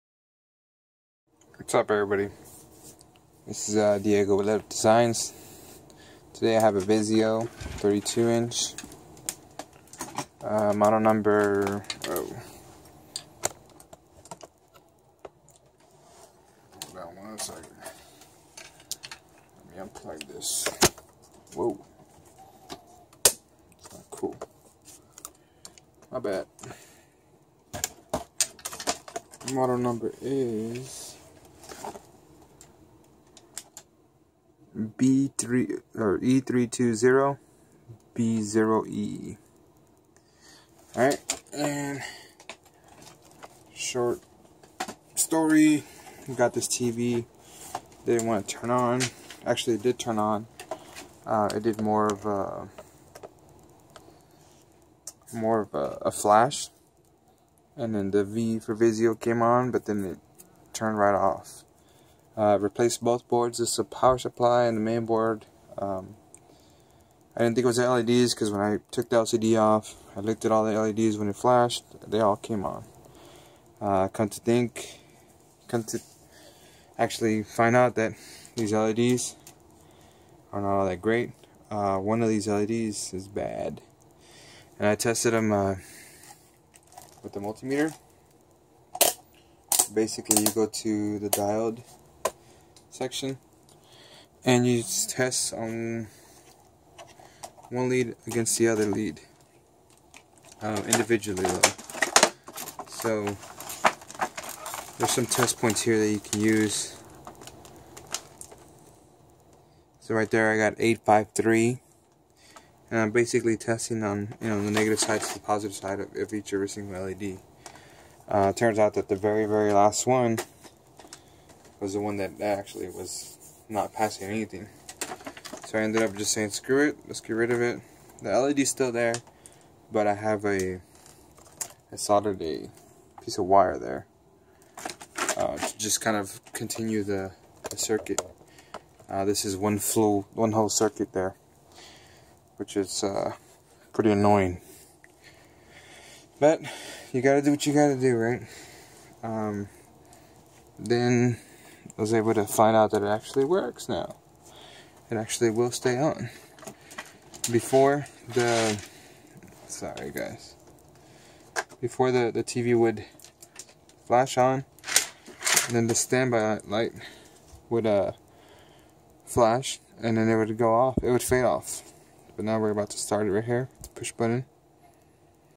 Vizio LED fix : does not turn on or turns off (not capacitors) no picture no power no backlight
Vizio led tv E320-B03 will work for others.. you just got to be patient.
LED TV TESTER

Sorry that video cut short... The leds finally turned and stayed on.. LCD works great.. even though i am missing one led its hardly noticeable.
What happens is that the power supply detects a short, or an open within the LED circuit and tells the main board. It then decides that to shut off as a safety.
Please let me know what yah think or if you have questions. Thanks again!